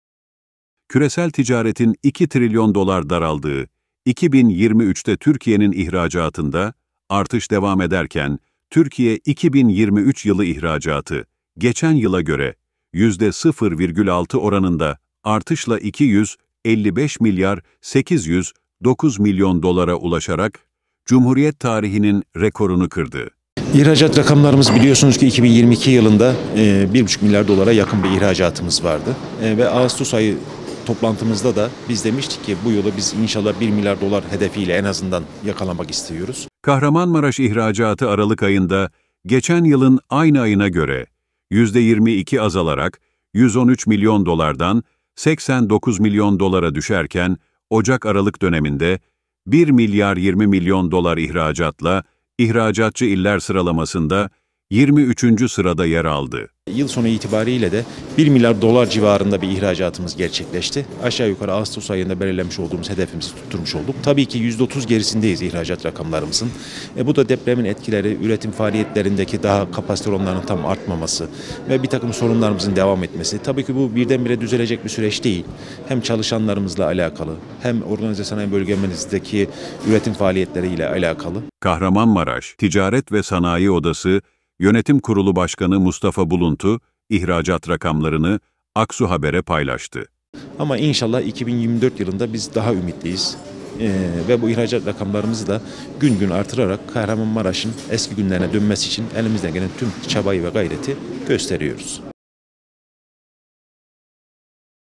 KMTSO Yönetim Kurulu Başkanı Buluntu’dan Aksu Habere Özel Açıklama
Küresel Ticaretin 2 Trilyon Dolar Daraldığı 2023'te Türkiye'nin İhracatında Artış Devam Ederken Türkiye 2023 Yılı İhracatı Geçen Yıla Göre Yüzde 0,6 Oranında Artışla 255 Milyar 809 Milyon Dolara Ulaşarak, Cumhuriyet Tarihinin Rekorunu Kırdı.
Kahramanmaraş İhracatı Aralık Ayında Geçen Yılın Aynı Ayına Göre %22 Azalarak 113 Milyon Dolardan 89 Milyon Dolara Düşerken Ocak-Aralık Döneminde 1 Milyar 20 Milyon Dolar İhracatla İhracatçı İller Sıralamasında 23. Sırada Yer Aldı
Kmtso Yönetim Kurulu Başkanı Mustafa Buluntu İhracat Rakamlarını Aksu Haber’e Paylaştı.